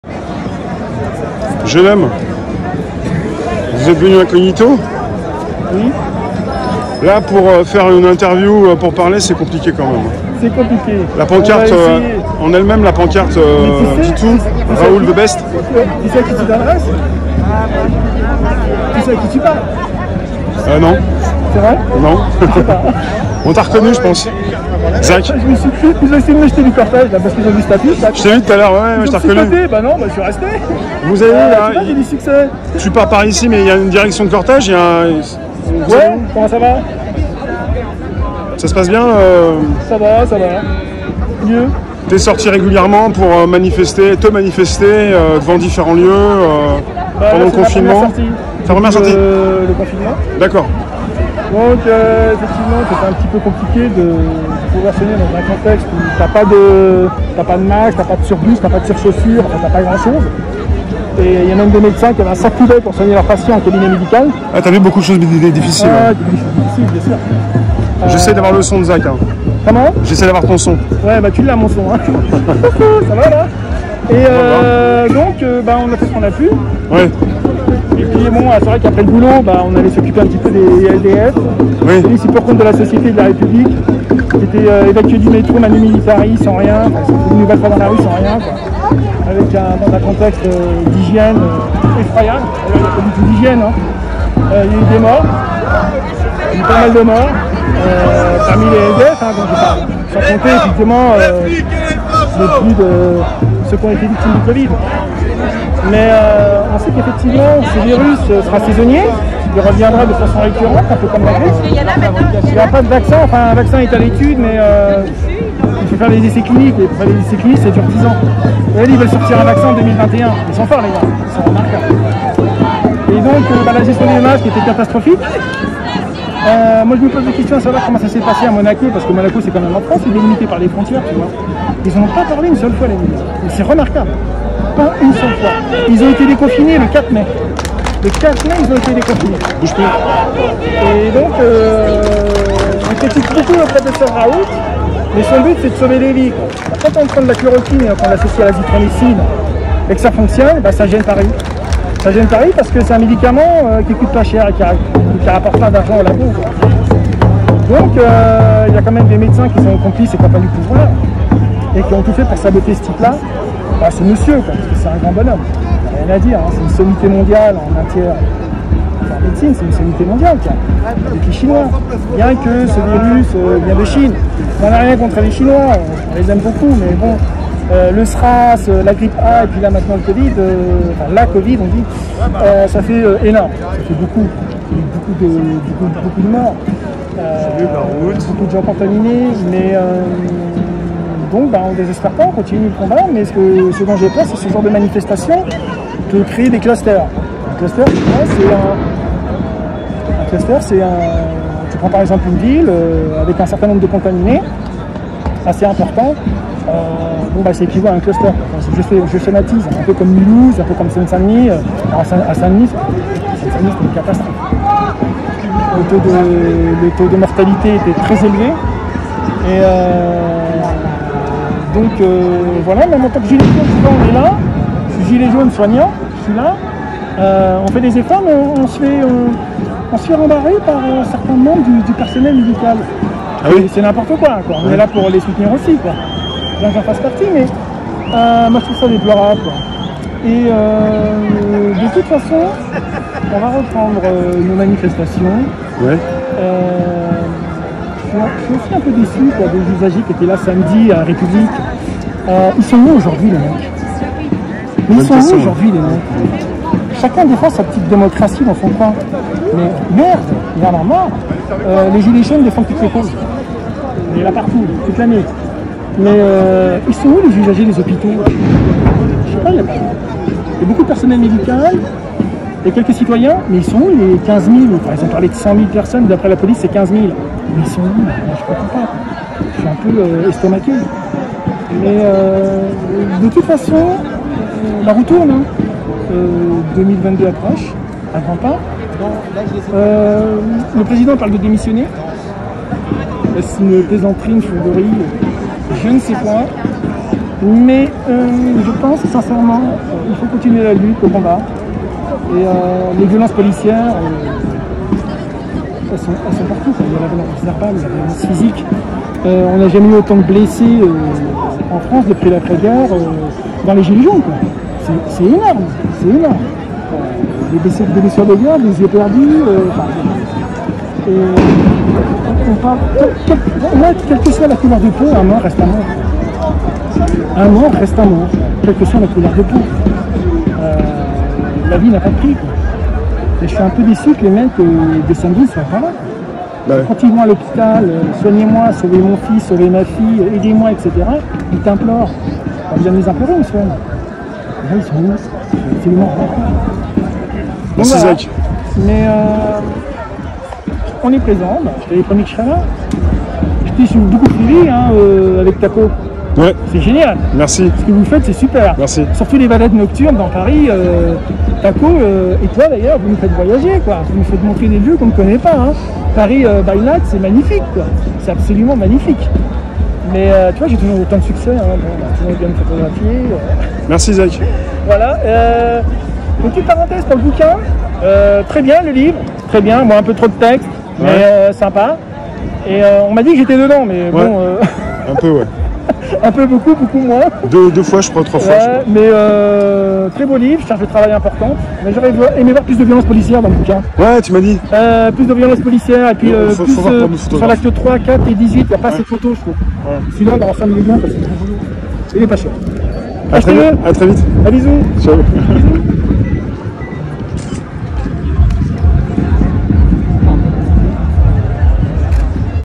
«Des médecins qui avaient des sacs poubelles pour soigner leur patient en cabinet médical»
«Il y avait des médecins qui avaient des sacs poubelles pour soigner leur patients en cabinet médical »

Mardi 16 Juin 2020